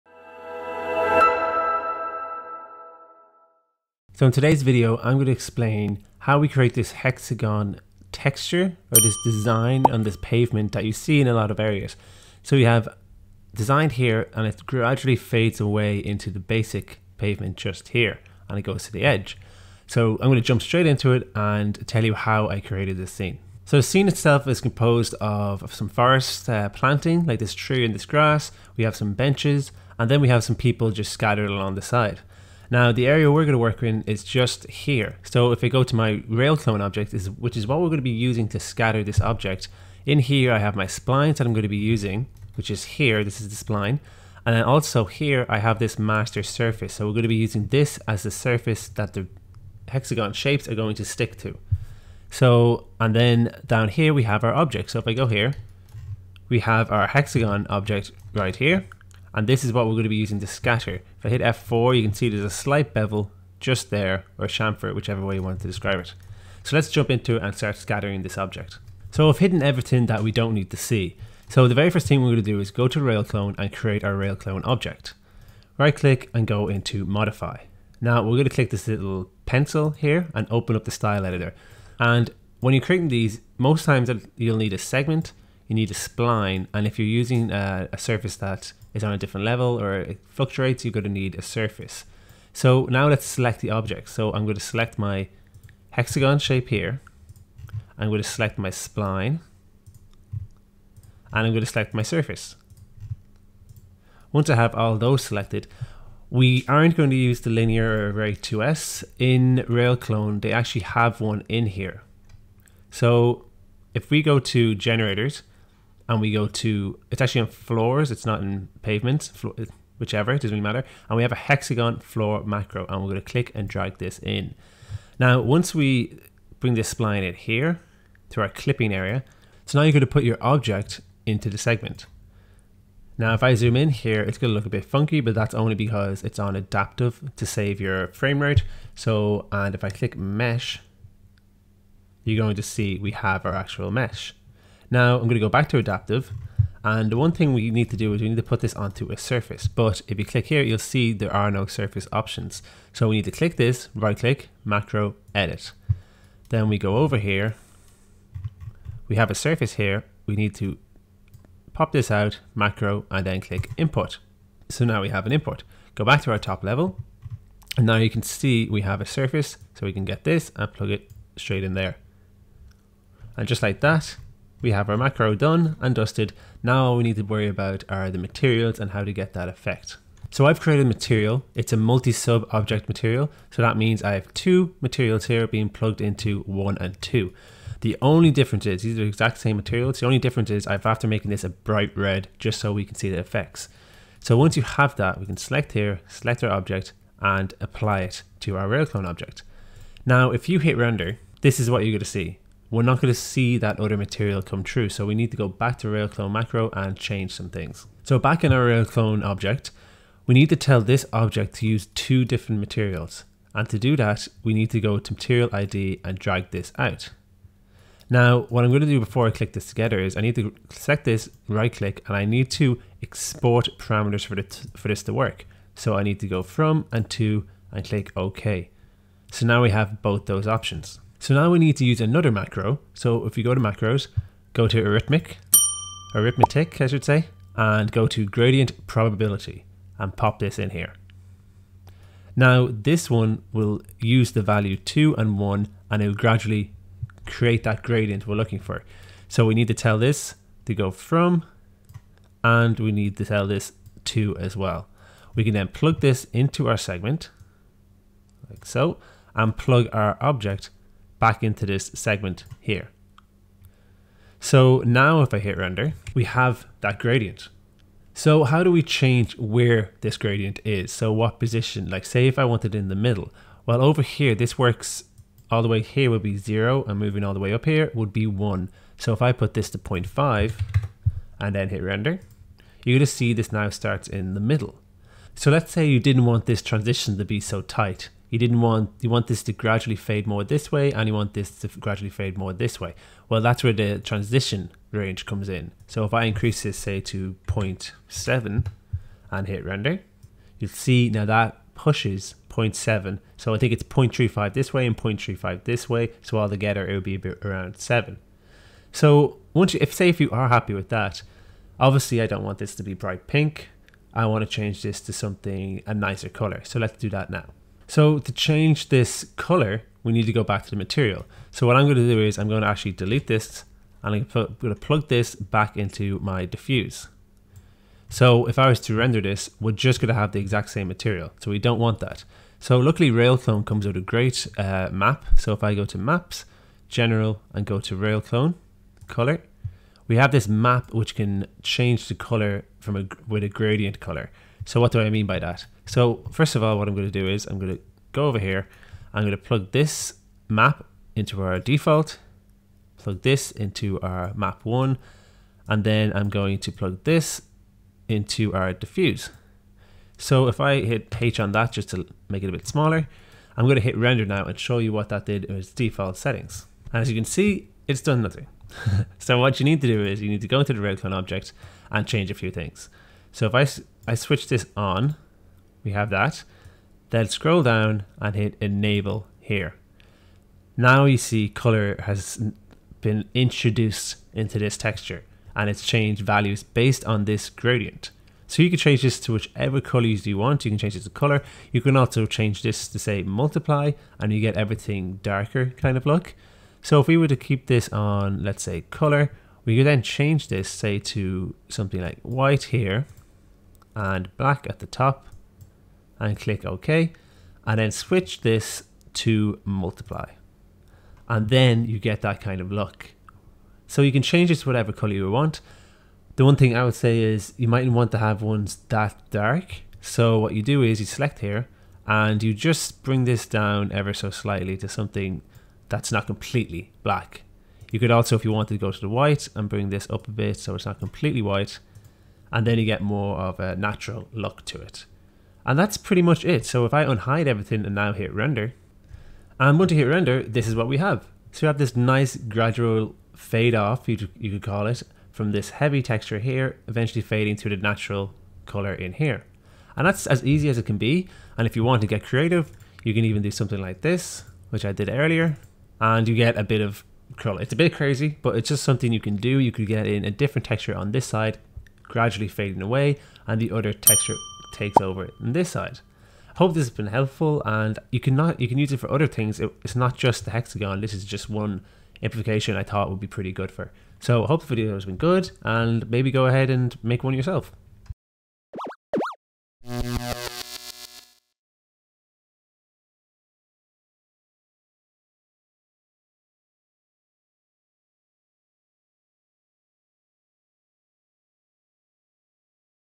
How To Create Pavement Patterns
Creating road materials can be tough but here I show you multiple ways to create your road materials using Vray Distance Tex and splines.

00:00 - Intro
00:23 - Scene Setup
01:10 - Creating Pavement Pattern
03:00 - Pavement Material
03:27 - Adding Pavement Effect
04:31 - Editing The Effect
05:18 - Refining Our Material
07:23 - Final Results